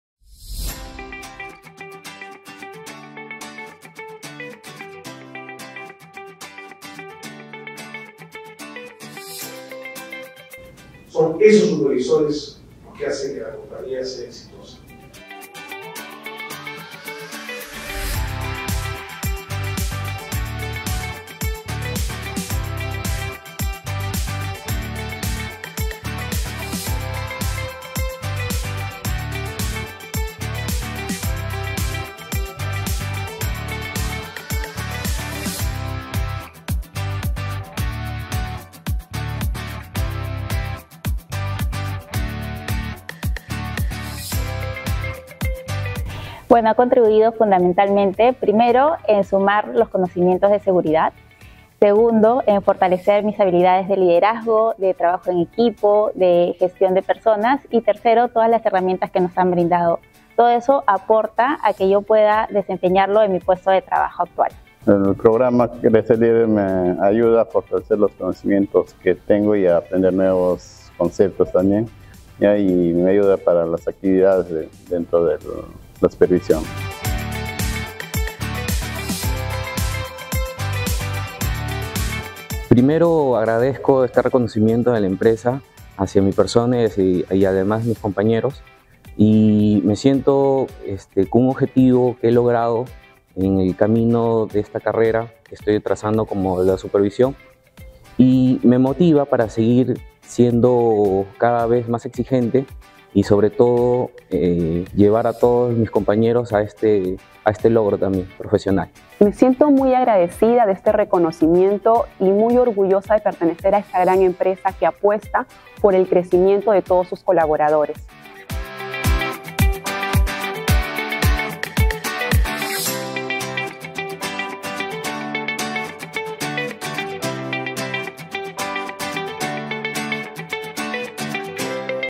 Programa Crece Líder
¡Enhorabuena a los brillantes supervisores Ferreyros que destacaron durante el 2023 el Programa Crece Líder! Reconocemos su dedicación y logros destacados de la primera y segunda promoción. Únete a nosotros para conocer más sobre sus inspiradoras historias y testimonios en el video. ¡Felicidades a todos los premiados! 👏